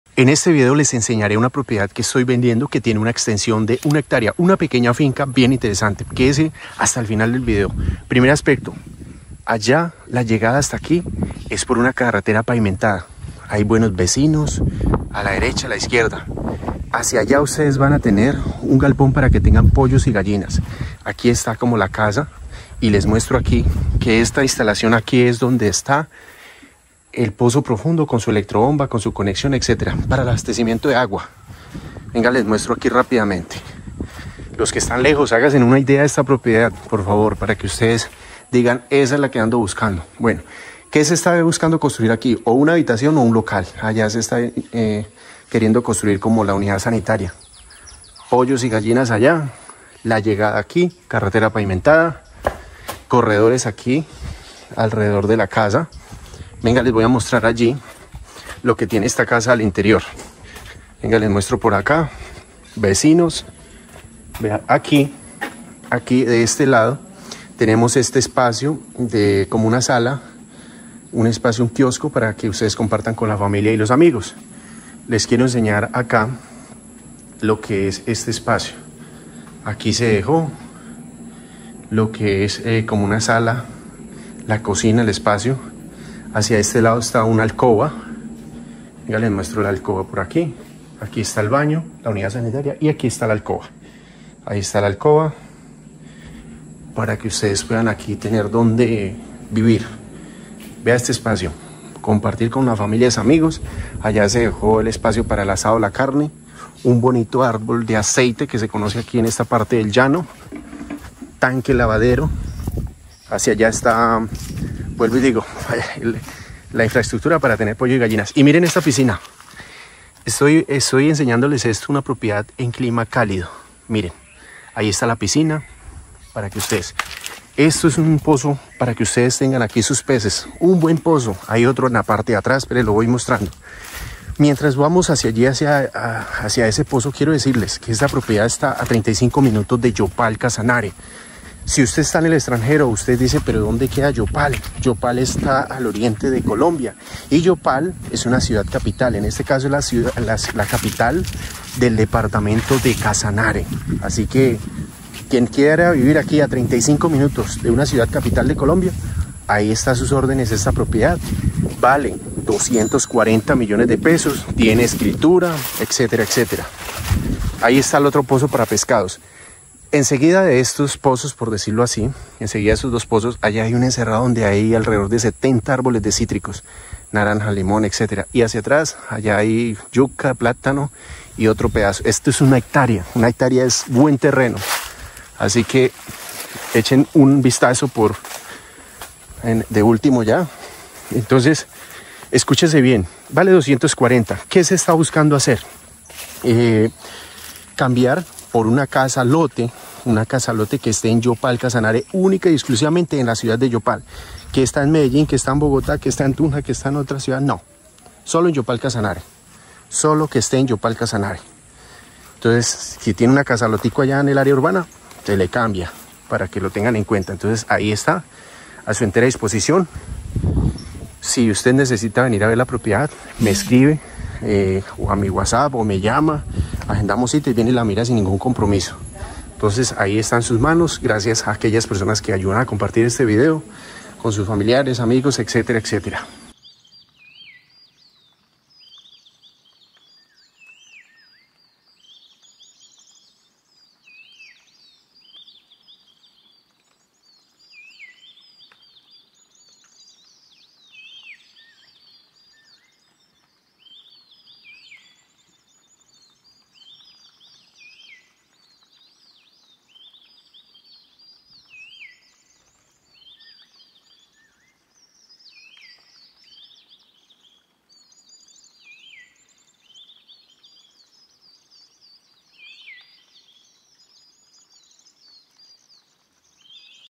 🔥PROPIEDAD VENDIDA🔥
🔥(SE VENDE)🔥FINCA PEQUEÑA DE (1) HECTÁREA🍃Se cambia por casa lote de MENOR VALOR, que esté únicamente en (Yopal)🍃

UBICACIÓN:
La finca está ubicada a (35) minutos (38 kms) del área urbana de la ciudad de (Yopal – Casanare). La vía de acceso toda es pavimentada.

Está ubicada saliendo desde la calle 40, pasando por la (Subasta ganadera de Casanare), siguiendo la vía (Matepantano), pasando por la universidad de la (Salle) y sigue bajando hacia la vía (Punto Nuevo). Exactamente está en la vereda (Barbillal).

COMPOSICIÓN – DETALLES:
El área total del predio es de: (1) hectárea, cuenta con escritura pública y su respectivo certificado de libertad y tradición. Está a unos (350 m.s.n.m), el clima de la región es cálido.

De infraestructura tiene (1) edificación de (1) habitación, (1) baño, cocina, sala en espacio abierto, área para local, (1) piscina, (1) encerrado para gallinas, (2) pozos amplios para tener peces, (70) árboles de cítricos y demás.

Se cuenta con línea de energía eléctrica, línea de gas domiciliario y para el abastecimiento del agua se tiene (1) pozo profundo propio.

EL PRECIO ES DE:
COP🇨🇴: $240´000.000
Recuerden que se recibe casa lote de MENOR VALOR, que esté únicamente en (Yopal).